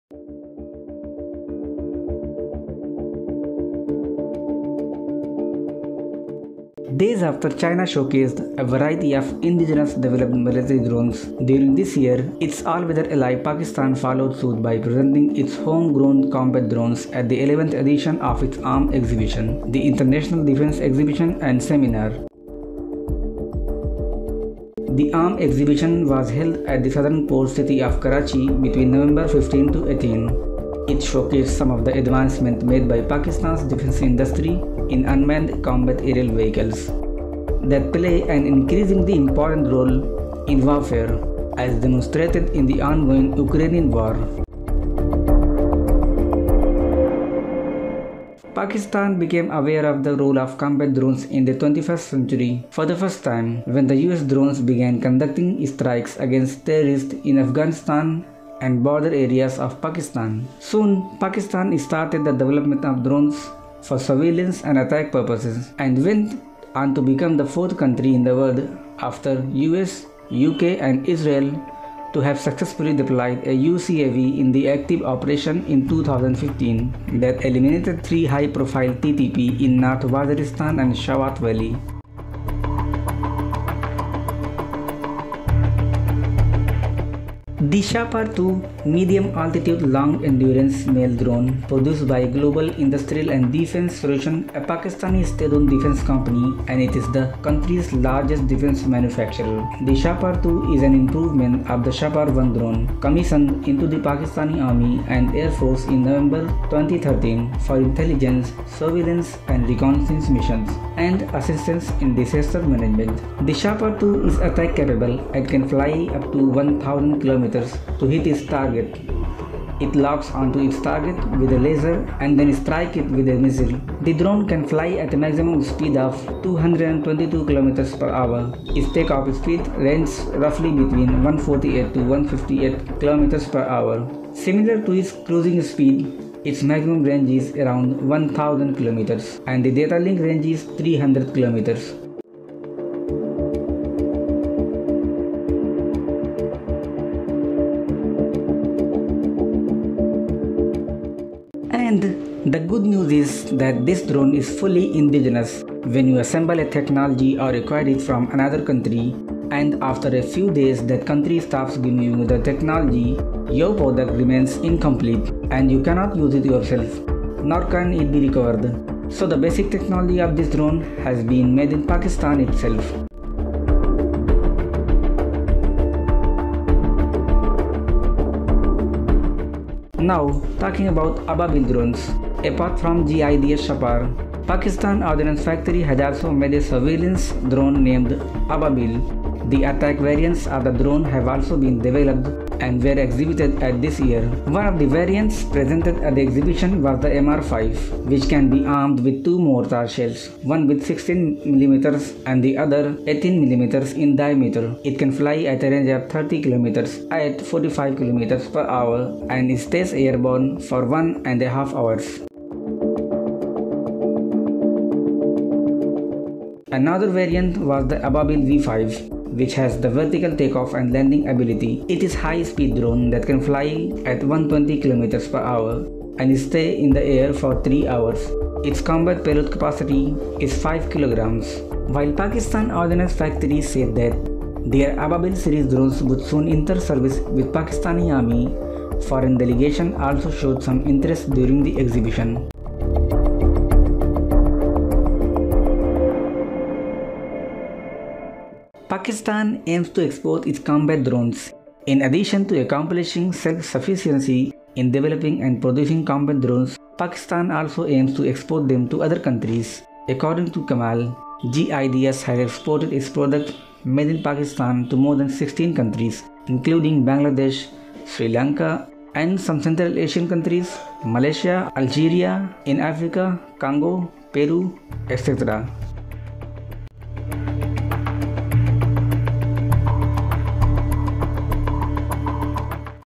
Will Pakistan Be Next Drone Power ? || Defense Updates
2:20 Indigenous Manufacturing
2:53 Ababeel Drone
4:03 Drone Export

Days after China showcased a variety of indigenous developed military drones during this year’s  Its all-weather ally Pakistan followed suit by presenting its homegrown combat drones at the 11th edition of its arms exhibition, The International Defense Exhibition and Seminar

The arms exhibition was held at the southern port city of Karachi between November 15-18. It showcased some of the advancements made by Pakistan’s defense industry in unmanned combat aerial vehicles (UCAVs) That play an increasingly important role in warfare, as demonstrated in the ongoing Ukraine war.

Pakistan became aware of the role of combat drones in 21st-century warfare for the first time when u.s drones began conducting strikes against terrorist in Afghanistan and border areas of Pakistan.

Soon, Pakistan started the development of drones for surveillance and attack purposes and went on to become the fourth country in the world after the US, UK & Israel to have successfully deployed a UCAV in an active operation in 2015 That eliminated three high-profile  (TTP)  in North Waziristan’s Shawat Valley.

Shahpar-2 UCAV
The Shahpar-2 Medium Altitude Long Endurance (MALE) drone produced by Global Industrial and Defense Solutions a Pakistani state-owned defense company and it is the country’s largest defense manufacturer

The Shahpar-2 is an improvement of the Shahpar-1 drone commissioned into the Pakistan Army and Air Force in November 2013 for intelligence, surveillance, and reconnaissance missions and assistance in disaster management.

The Shahpur-2 is attack-capable and can fly up to 1000 kilometers to hit its target. It locks onto its target with a laser and then strikes it with a missile.

The drone can fly at a maximum speed of around 222 kilometers per hour (kph). Its take-off speed ranges roughly between 148 to 158 kph, similar to its cruising speed. Its maximum range is around 1,050 kilometers, and the data link range is 300 kilometers.

The Shahpur-2 can restart its engine during flight and contact satellites in any campaign, day or night.

And the good news is that this drone is fully indigenous
when you assemble a technology or acquire it from another country, and after a few days that country stops giving you the technology, your product remains incomplete, and you cannot use it yourself. Nor can it be recovered, so the basic technology of this drone has been made in Pakistan itself,”

Talking About Ababeel Drones

Apart from GIDS’ Shahpur, Pakistan’s Ordnance Factory had also made a surveillance drone named ‘Ababeel.’ The attack variants of the drone have also been developed and were exhibited at this year.

One of the variants presented at the exhibition was the MR-5 drone which can be armed with two mortar shells, one with 16 millimeters and the other 18 millimeters in diameter. It can fly at a range of 30 kilometers at 45 kilometers per hour and stays airborne for one and a half hours.

Another variant was the Ababeel V-5 which has the vertical take-off and landing  ability. It is a high-speed drone that can fly at 120 kilometers per hour and stay in the air for 3 hours. Its combat payload carrying capacity is five kilograms.

Pakistan Aims To Export Its Combat Drones
In addition to accomplishing self-sufficiency in developing and producing combat drones, Pakistan also aims to export them to other countries.

According to Kamal, GIDS has exported its products made in Pakistan to more than 16 countries, including Bangladesh, Sri Lanka, central asian contries, Malaysia, Algeria in Africa, Congo, Peru, etc.

While Pakistan Ordnance Factory said their Ababeel series drones would soon enter service with Pakistan Army, foreign delegations also showed some interest during the exhibition.

So far, the Pakistan Ordnance Factory has sold weapons worth more than 30 million us dollars to more than forty countries, according to its spokesperson.